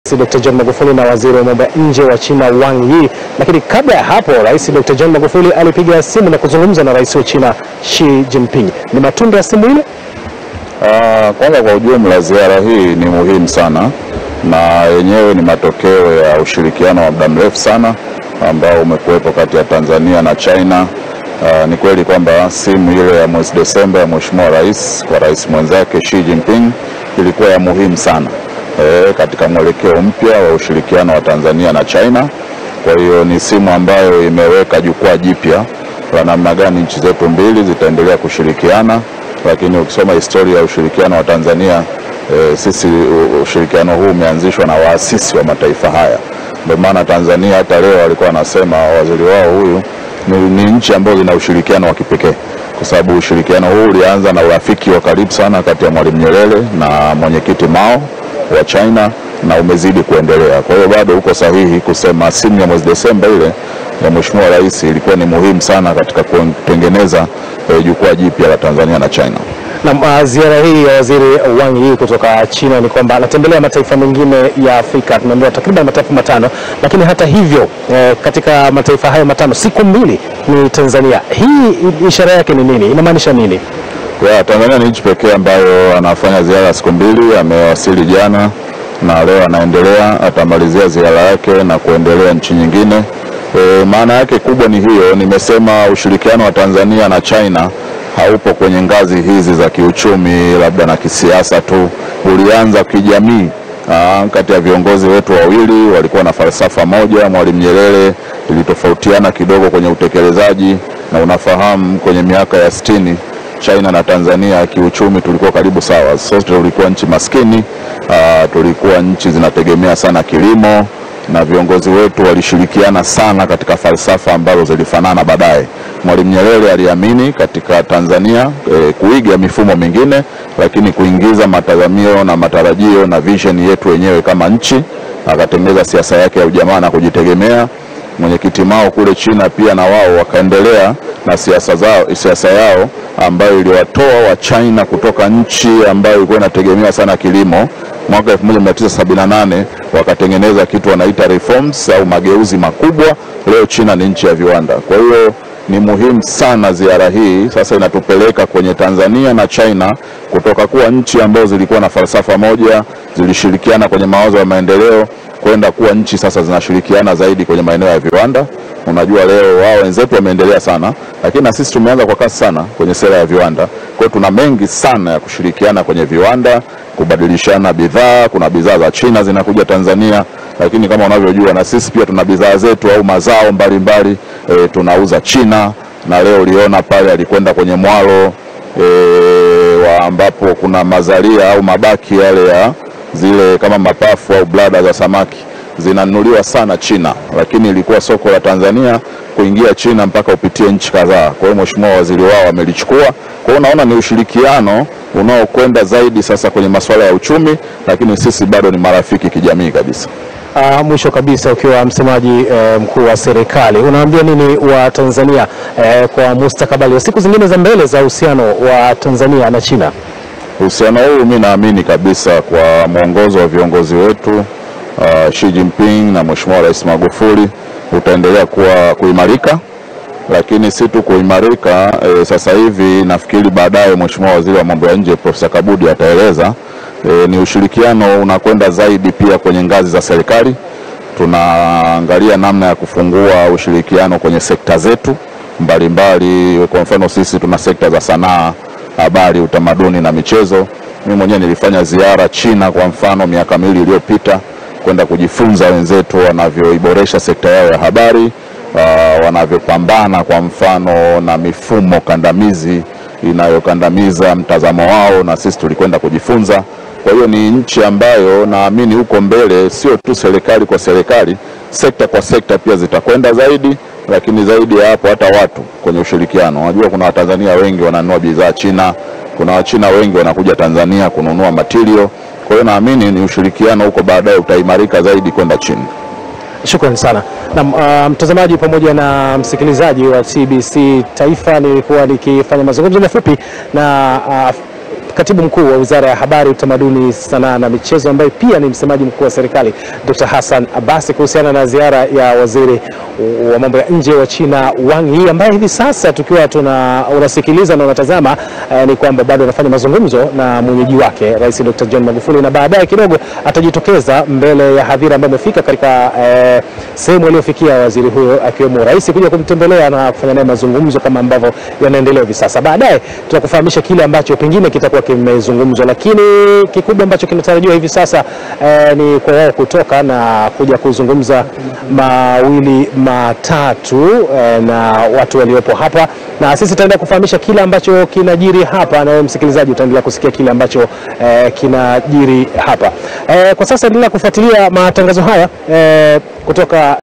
"SIMU ya RAIS MAGUFULI na RAIS wa CHINA ILIKUWA ni MUHIMU" - DKT ABBAS Afunguka..
MSEMAJI Mkuu wa Serikali, Dkt Hassan Abbas, ameelezea ujio wa Waziri wa Mambo ya Nje wa China, Wang Yi, ambapo amesema ugeni huo ni matokeo ya mawasiliano ya simu yaliyofanyika baina ya viongozi wa nchi hizi mbili (Rais Magufuli na Rais wa China)